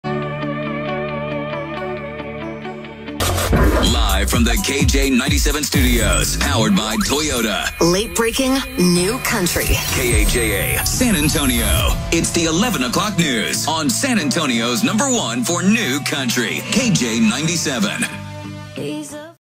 KJ97 San Antonio’s #1 For New Country (KAJA)
The Jingle Dude - KJ97 (KAJA) An IHeartRadio Station.

KAJA is a commercial FM radio station licensed to San Antonio, Texas. It airs a country music radio format and is owned by iHeartMedia, Inc. The studios and offices are located in the Stone Oak neighborhood in Far North San Antonio.